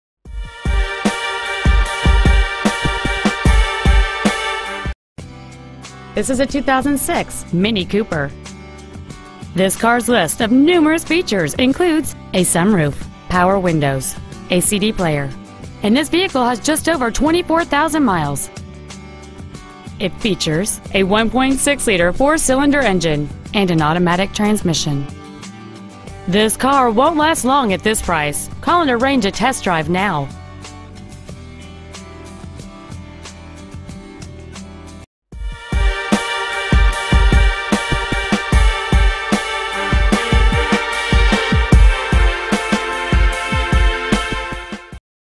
Pre-Owned 2006 Mini Cooper Burlington WA
We are proud to present this 2006 Mini Cooper.

 Year: 2006
 Make: Mini
 Model: Cooper
 Engine: 1.6L I4 OHC
 Trans.: Automatic
 Exterior: Red
 Miles: 24,102
 Interior: Gray

 1615 S. Goldenrod Road

 2006 Mini Cooper Burlington WA